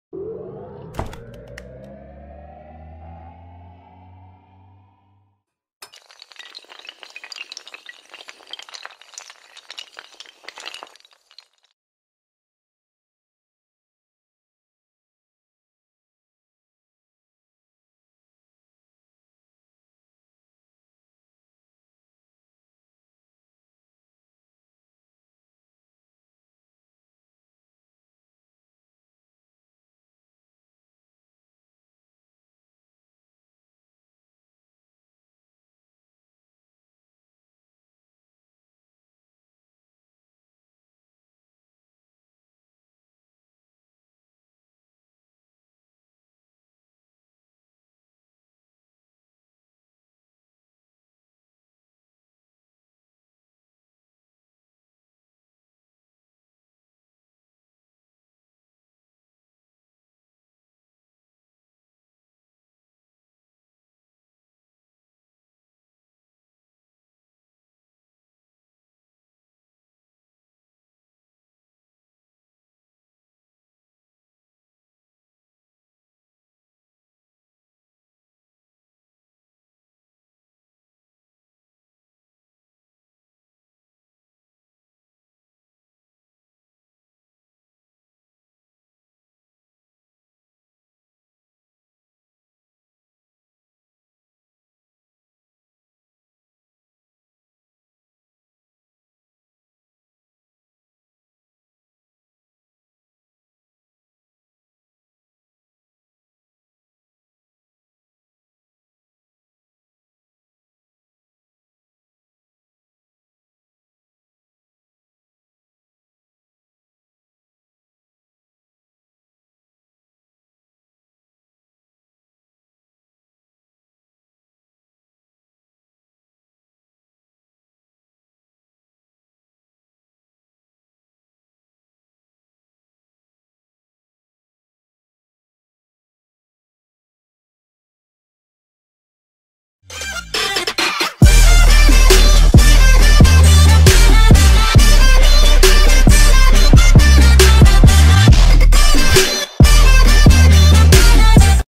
Copyright Free animated swimming fish Green Screen Effect | Chroma Key | Royalty Free
# Information about video :-Is video me aapko Copyright Free animated swimming fish Green Screen Effect milega jise aap apni video me bilkul free me use kar sakate hai.